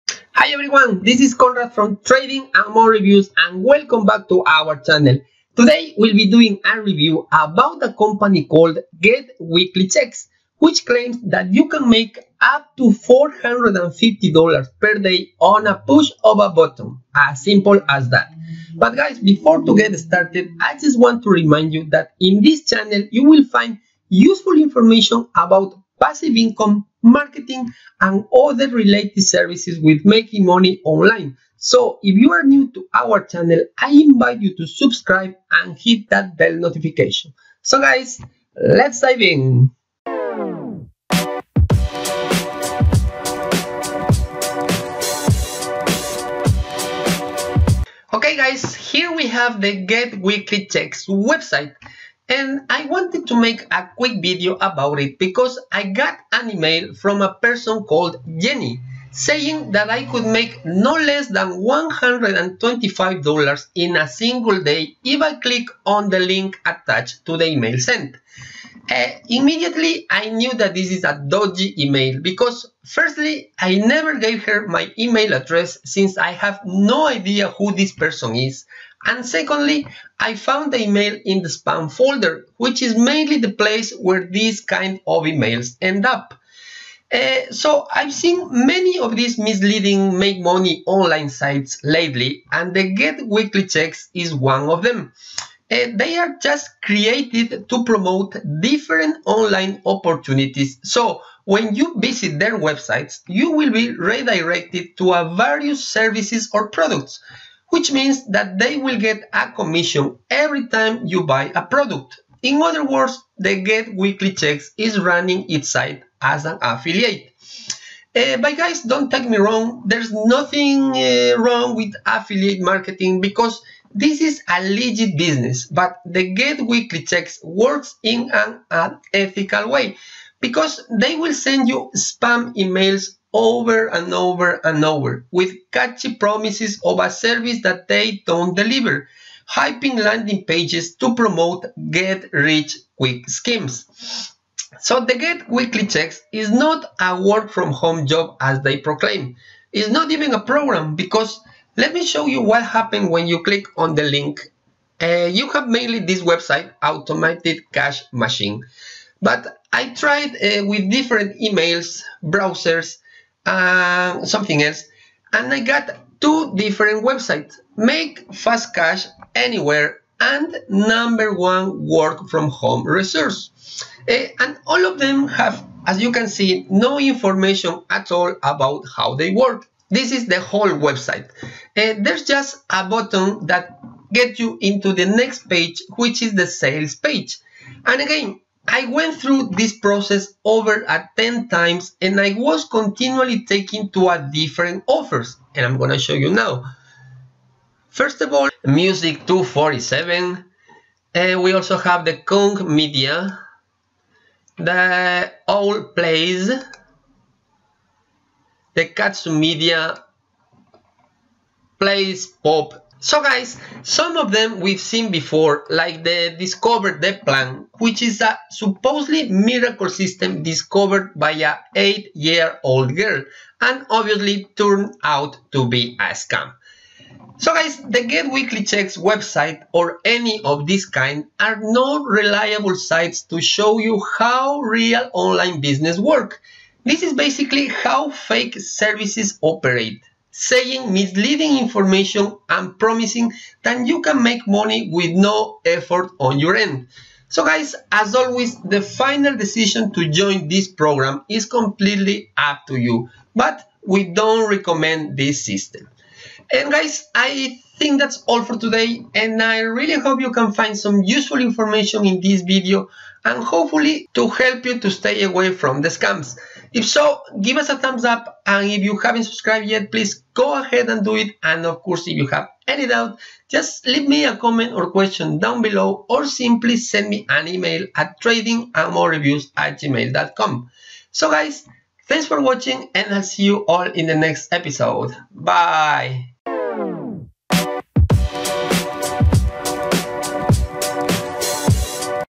Get Weekly Checks Review-Legit Service Or SCAM?
Today we'll be doing a review about a company called Get Weekly Checks which claims that you can make up to $450 per day on a push of a button, as easy as that! so I wanted to make a quick video about it because I got an email from a person called Jenny saying that I could make no less than $125 in a single day if I click on the link attached to the email sent.

Immediately I knew that this is a dodgy email because firstly I never gave her my email address since I have no idea who this person is, and secondly I found the email in the spam folder which is mainly the place where this kind of emails end up.

I hope this video is helpful, if so give us a thumbs up and if you haven't subscribe yet, please go ahead and do it, any doubt please just leave me a comment or question down below